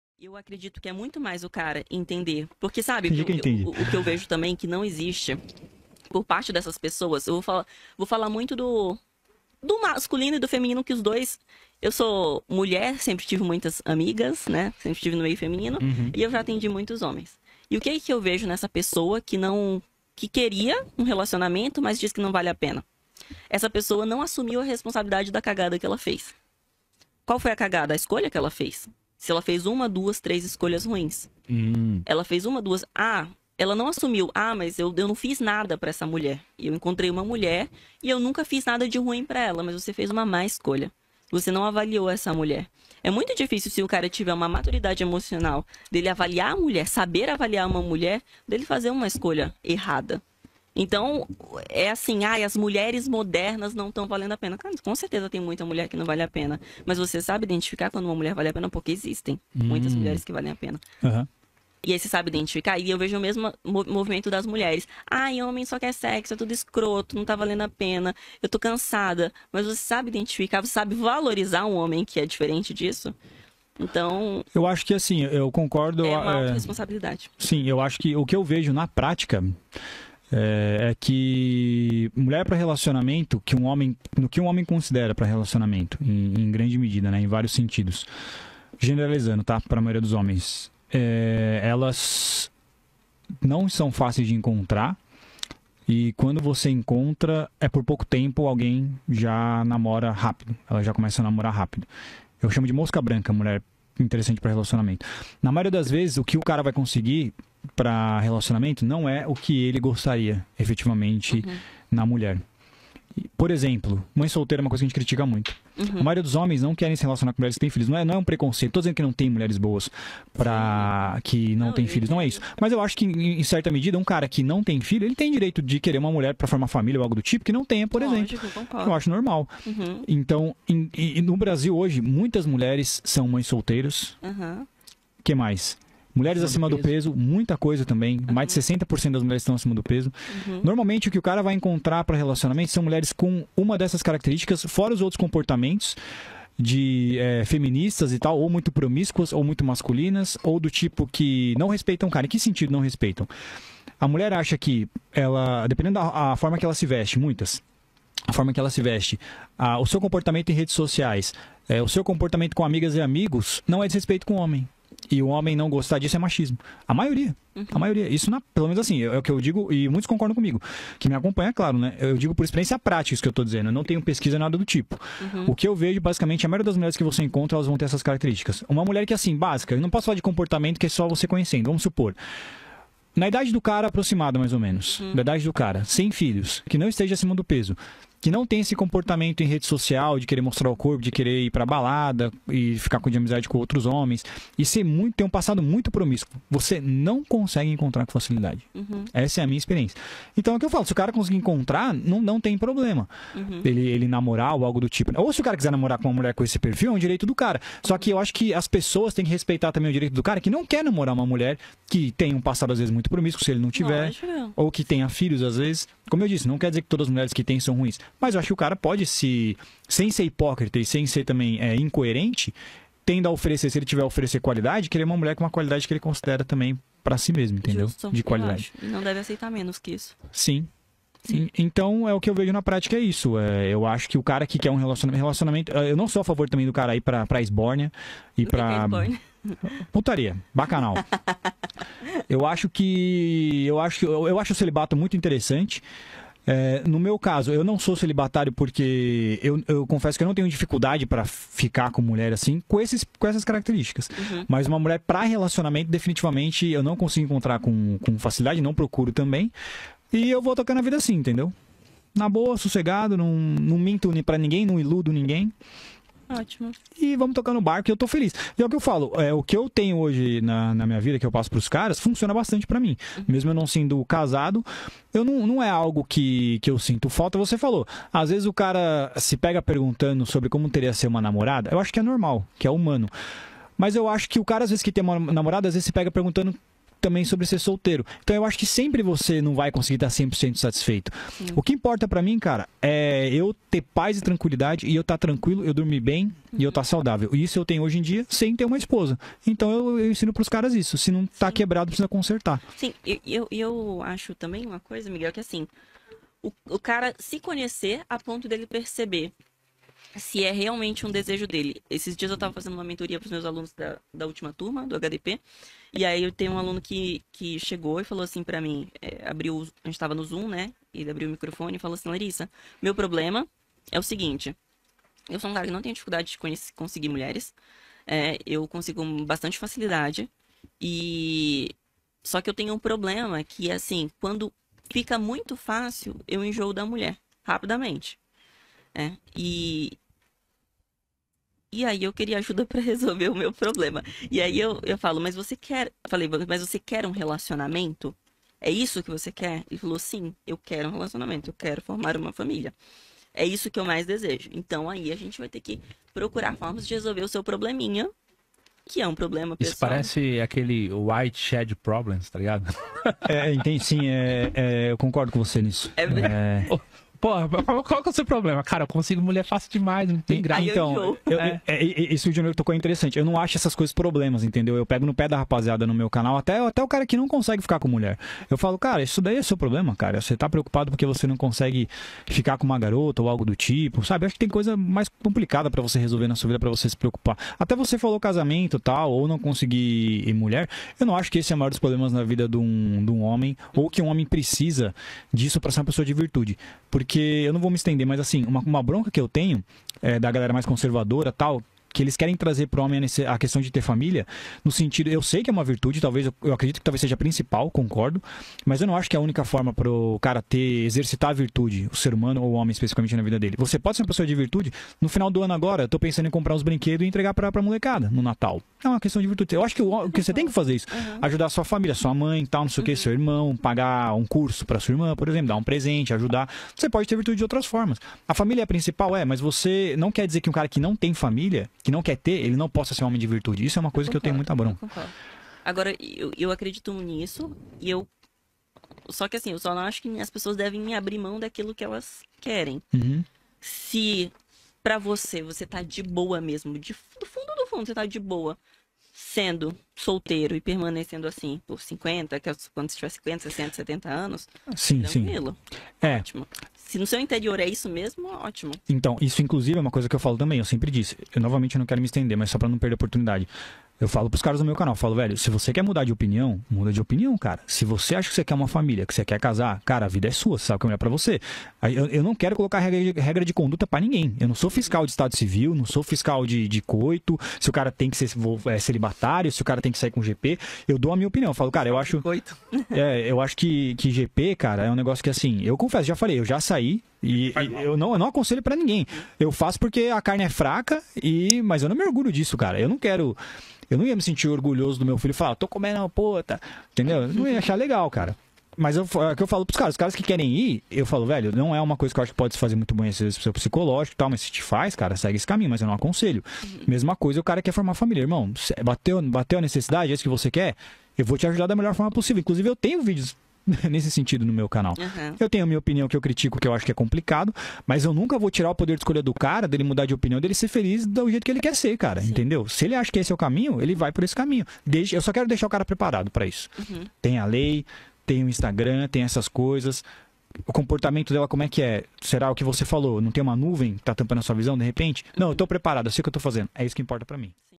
MULH3R "PARA RELACIONAMENTO" É MOSCA BRANCA | LARISSA ALENCAR | REDCAST
INSTAGRAM DOS ANFITRIÕES:
• Miguel: @AtitudeAlfa.Oficial